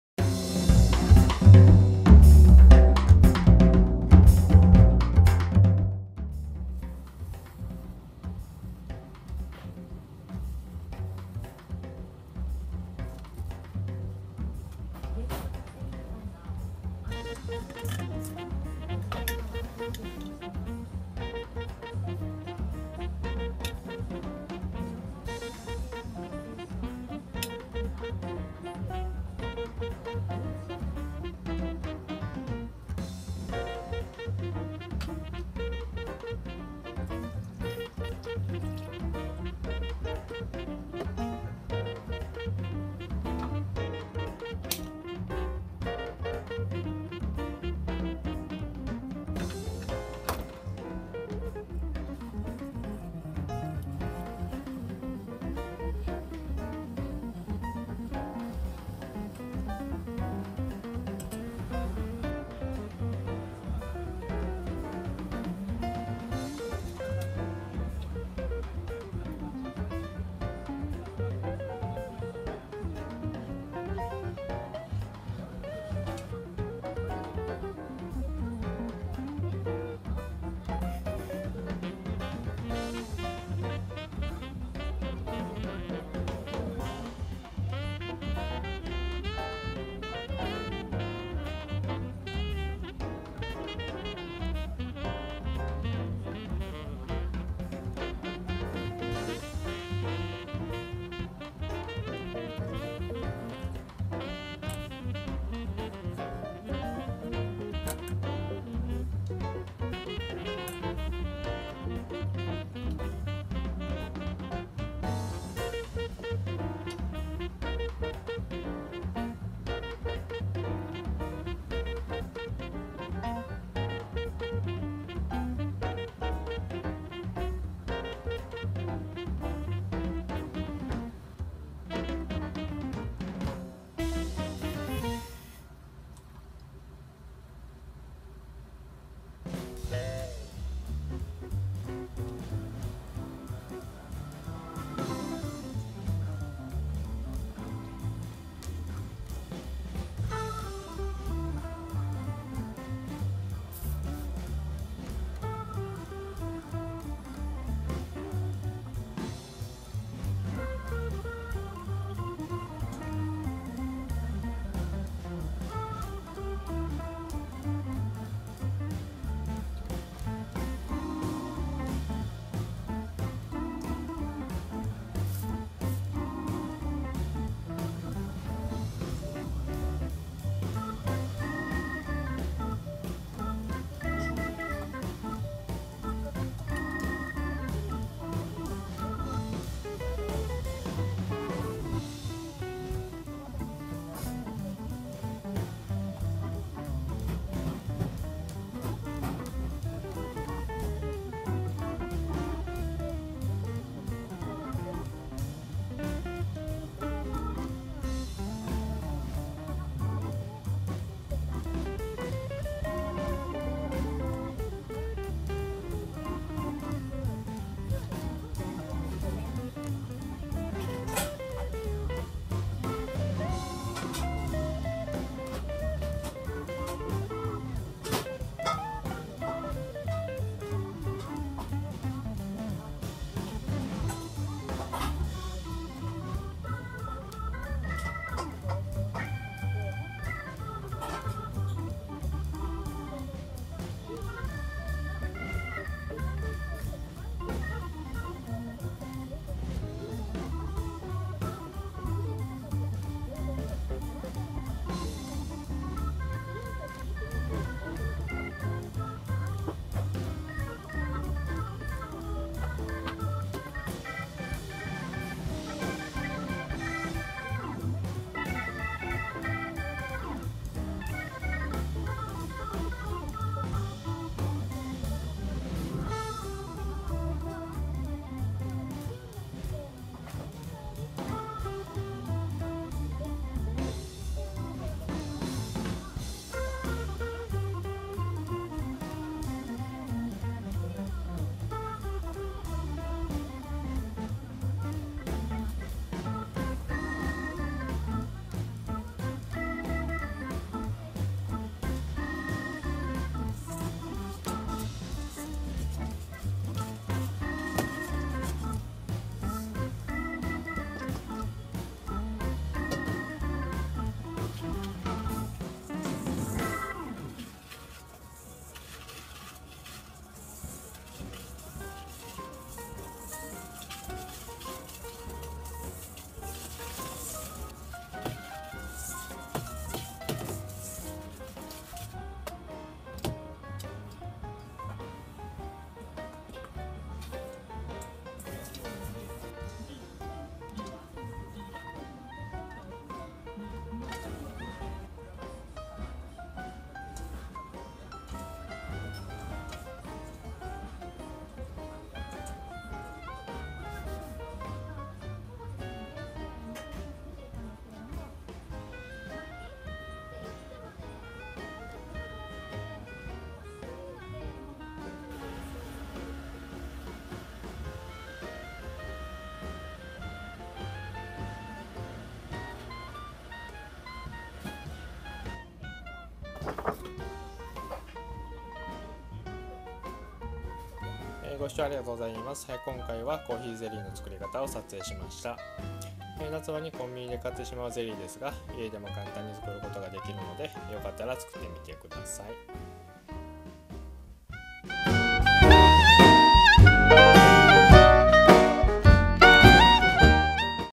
コーヒーゼリーの作り方。#69 マチの洋菓子工房！ Coffee Jelly Recipe
道具、材料の紹介。
森永 無塩バター

レシピ
インスタントコーヒー  10g
水  200g
冷水  35g
牛乳  100g
練乳  40g
水  100g

Instant coffee 10g
Water 200g
Granulated sugar 55g
Gelatin 7g
Cold water 35g
Milk 100g
Condensed milk 40g
Water 100g